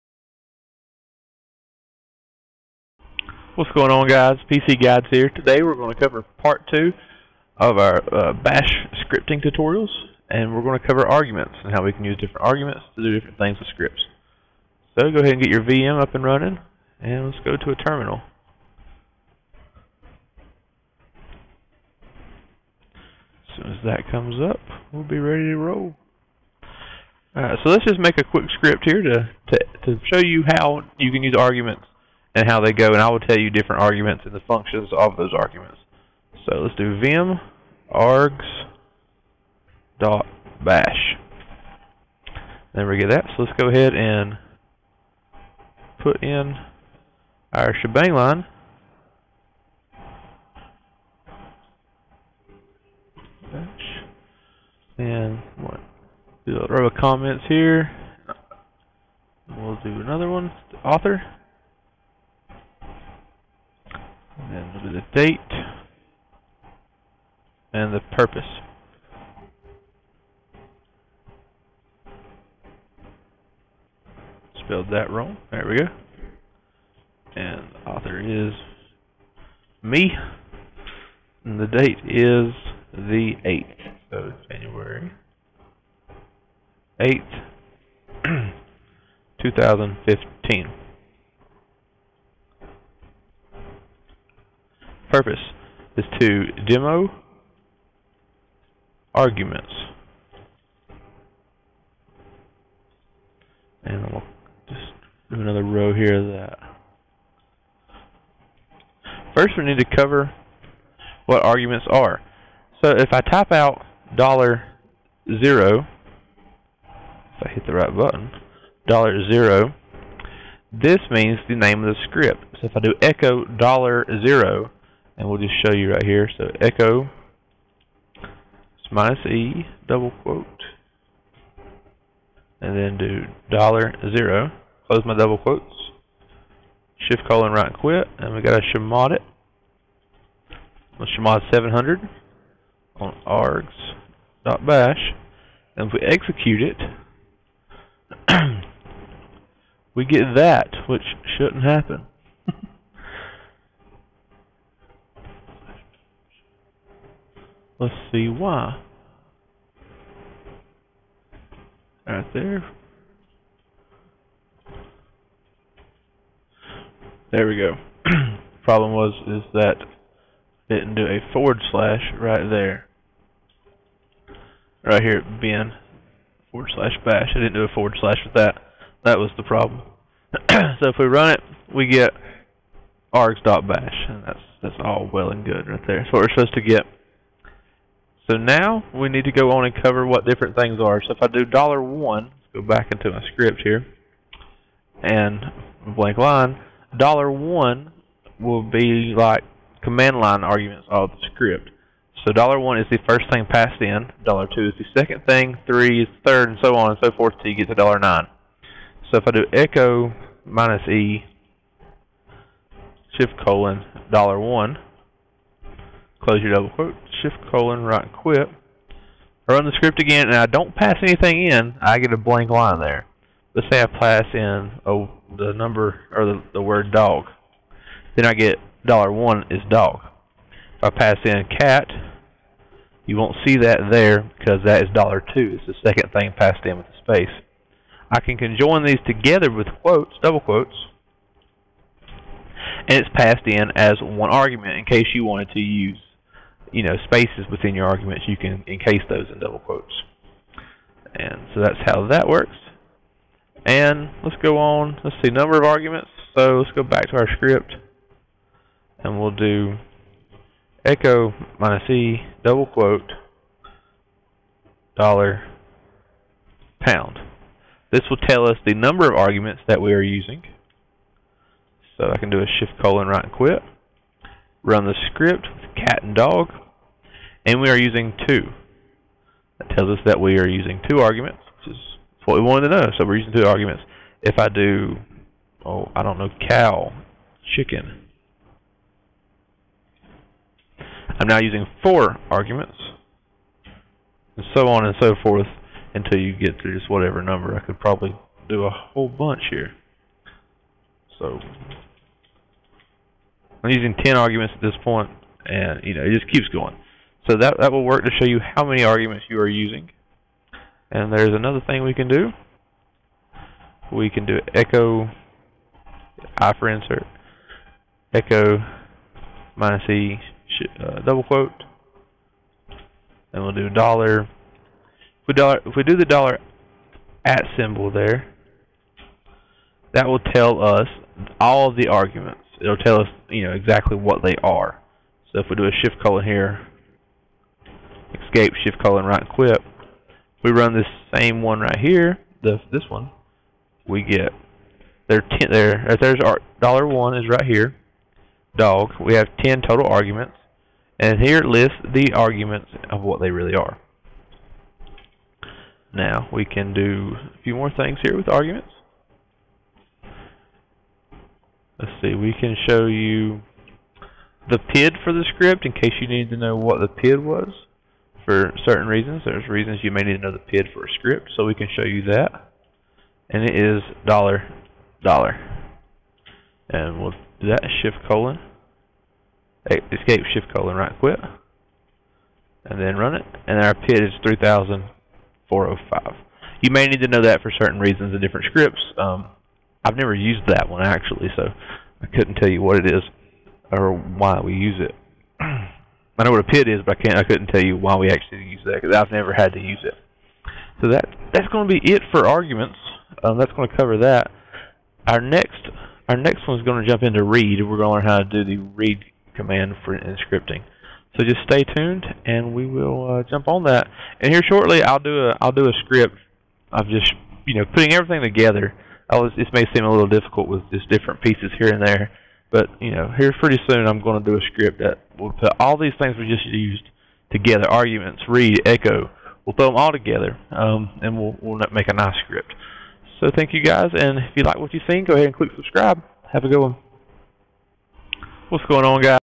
Bash scripting using arguments
This video will show users how to use arguments in bash scripting